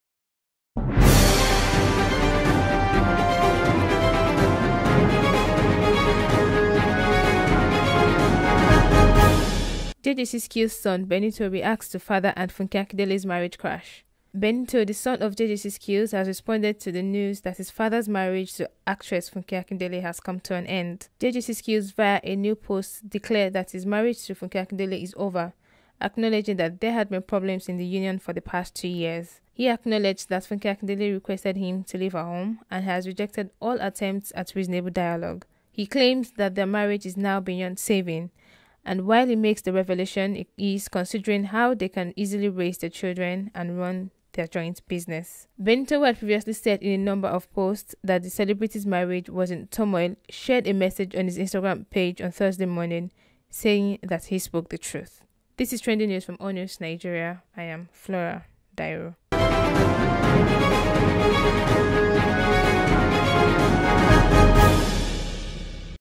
JJC Skillz's Son, Benito Reacts To Father, Funke Akindele's Marriage Crash
Benito, the son of JJC Skillz, has responded to the news that his father's marriage to actress Funke Akindele has come to an end.

JJC Skills via a new post declared that his marriage to Funke Akindele is over, acknowledging that there had been problems in the union for the past two years.

He acknowledged that Funke Akindele requested him to leave her home and has rejected all attempts at reasonable dialogue.